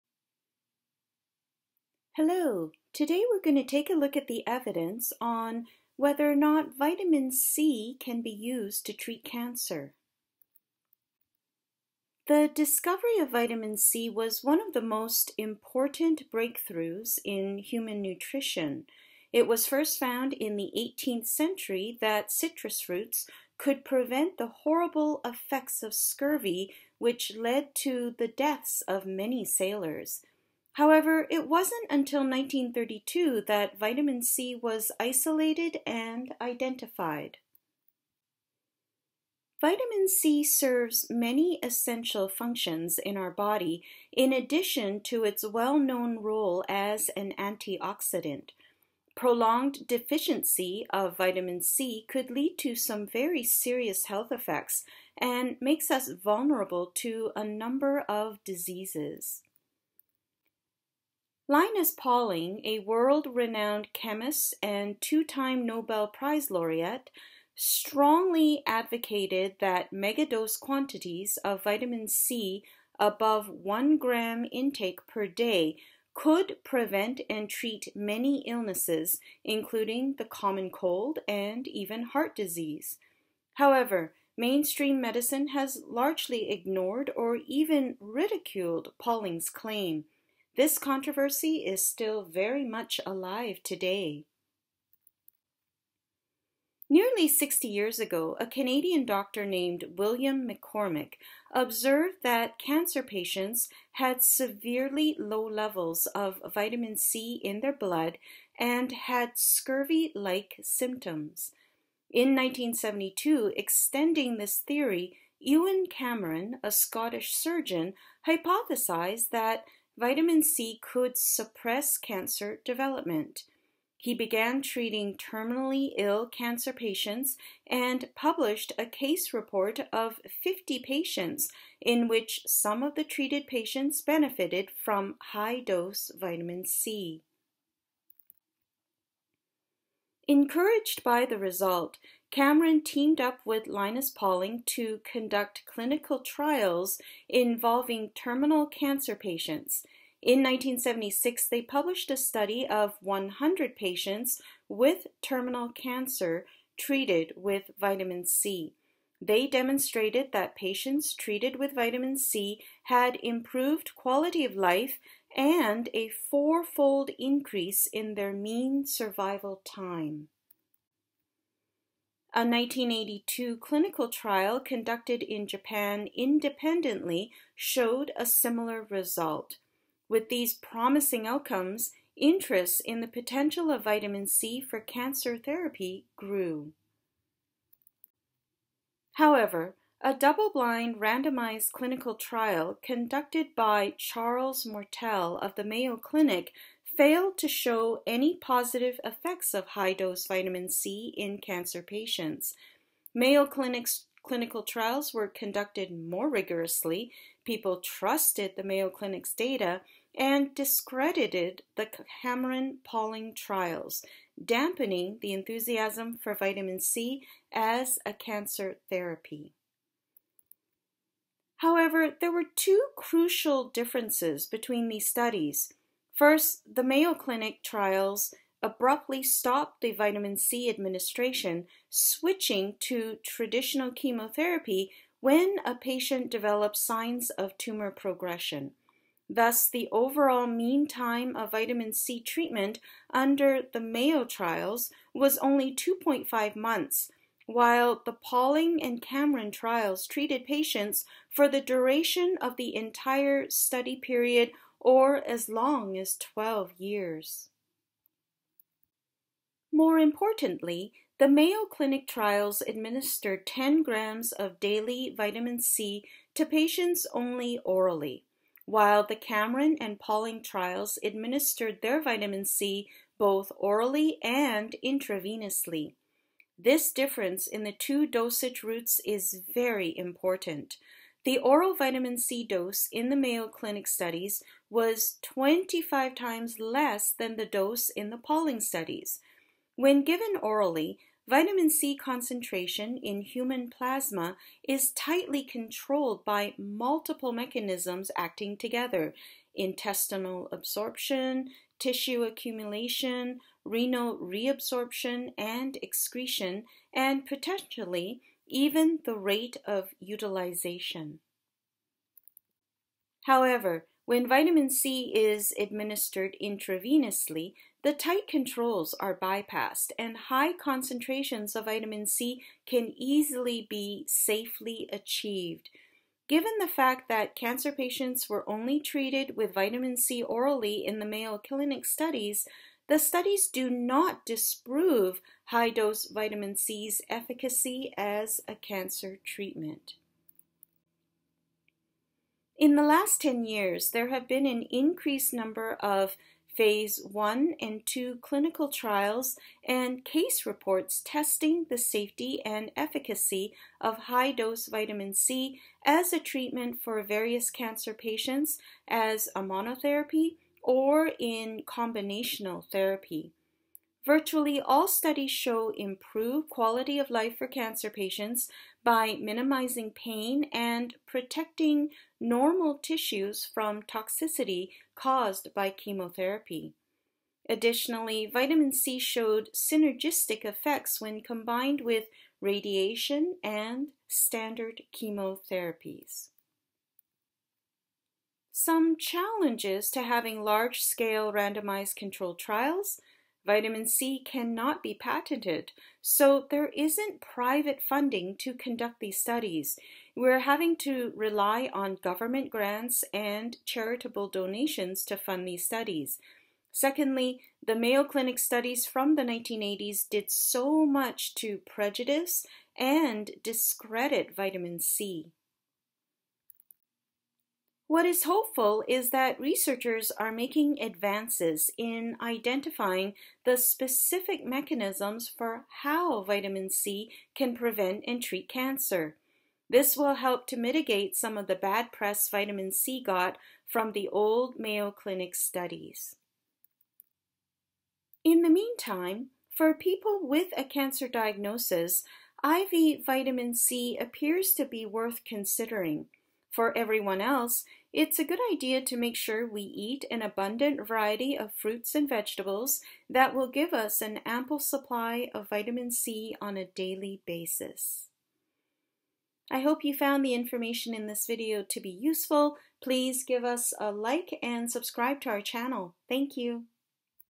Can Vitamin C Treat Cancer?
The use of high-dose vitamin C to treat cancer is a controversial topic. Today, we are taking a look at the scientific evidence on this idea.

Vitamin C has many essential functions in our body in addition to its well-known role as an antioxidant. Prolonged deficiency of vitamin C could lead to some serious health effects and make us vulnerable to a number of diseases.